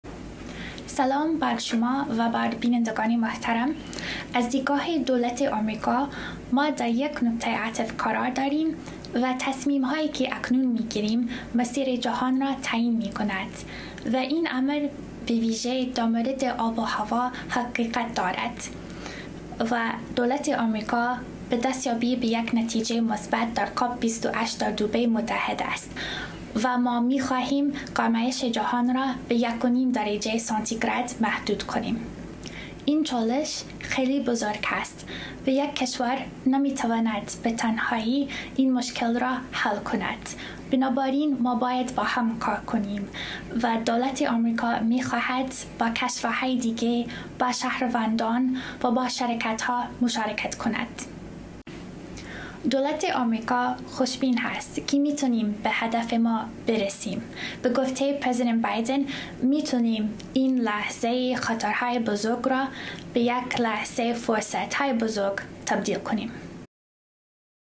سخنگوی فارسی‌زبان وزارت خارجه آمریکا:می‌خواهیم گرمایش جهان را به یک‌ونیم درجه سانتی‌گراد محدود کنیم
الیزابت استیکتی، سخنگوی فارسی‌زبان وزارت امور خارجه آمریکا در گفت‌وگو با ایران اینترنشنال: ما می‌خواهیم گرمایش جهان را به یک‌ونیم درجه سانتی‌گراد محدود کنیم